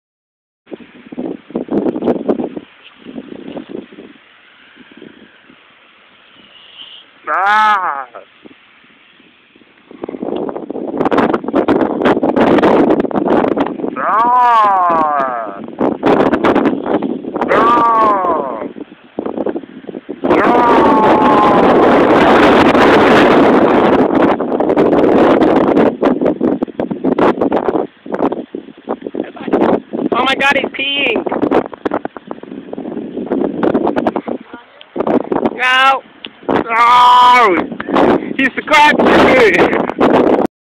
Alligator Snapper Turtle
Snappter Turtle pee's on my hand and scratches me.